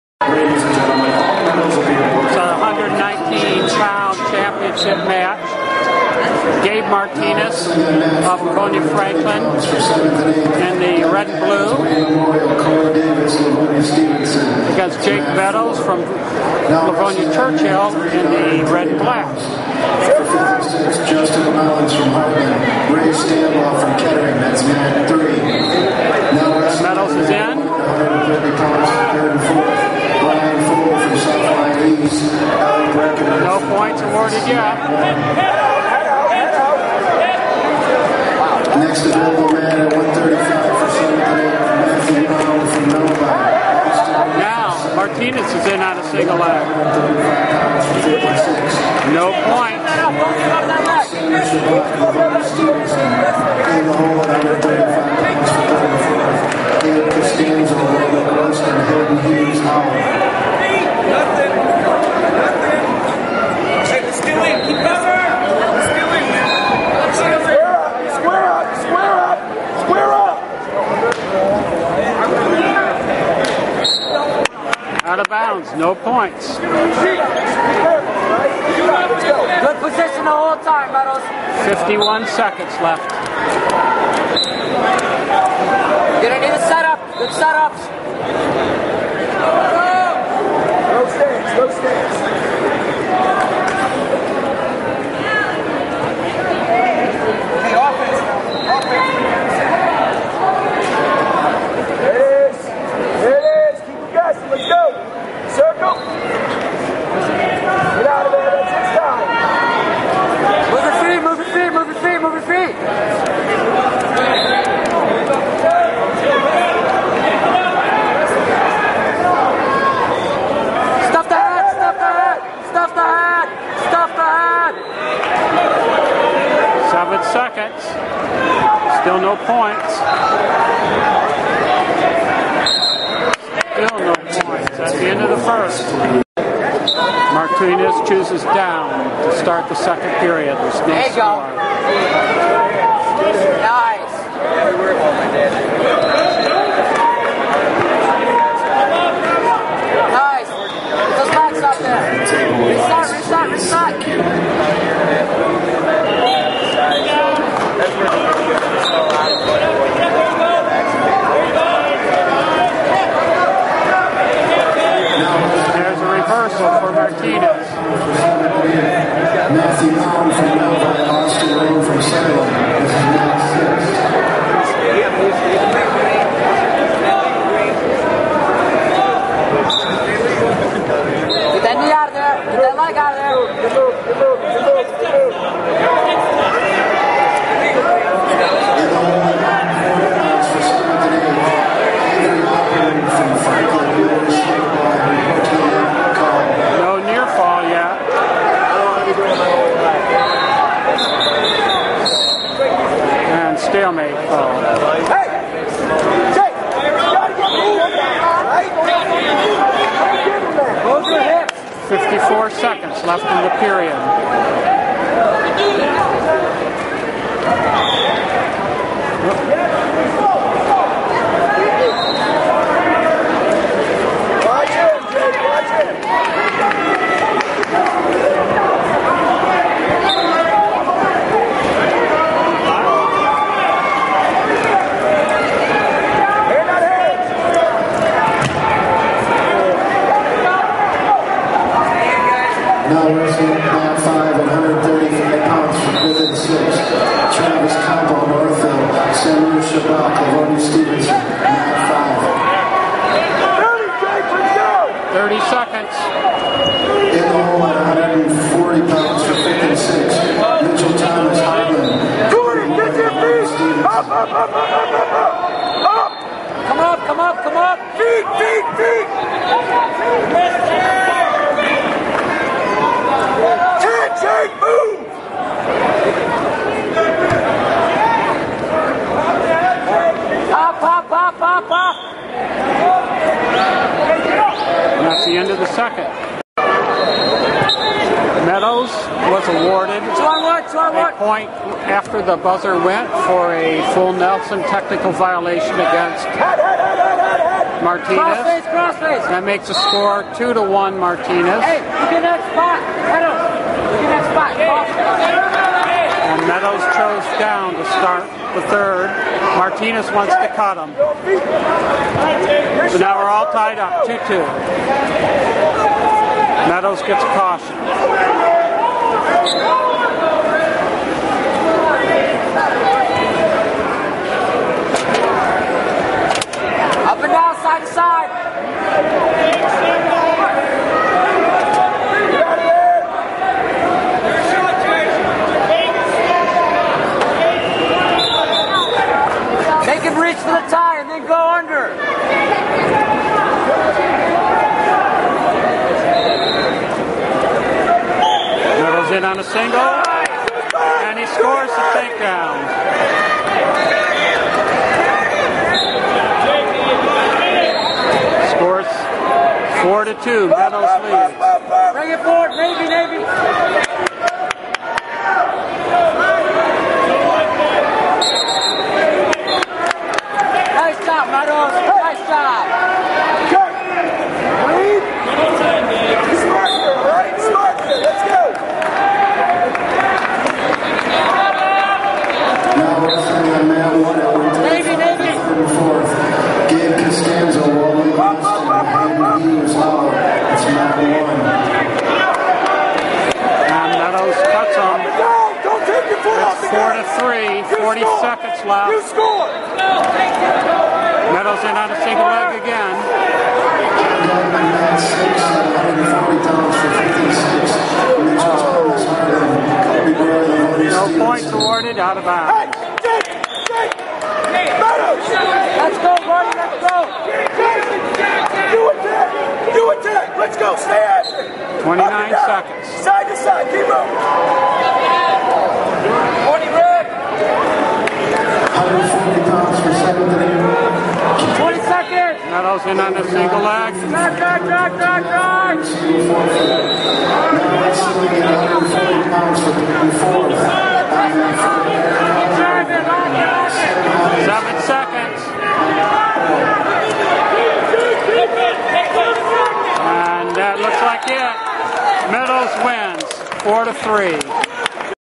Meadows vs Martinez
Jacob Meadows (Livonia Churchill) vs Gabe Martinez (Livonia Franklin) in the 119 pound finals.  KLAA Tournament, February 5, 2011.